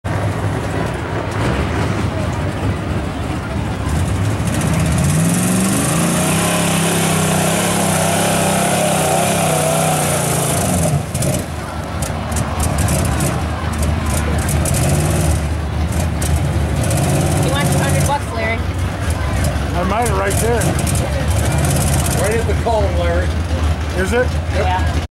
Dustin Boyer at Eaton Co Fair
Dustin Boyer driving at Eaton Co Fair on July 16, 2016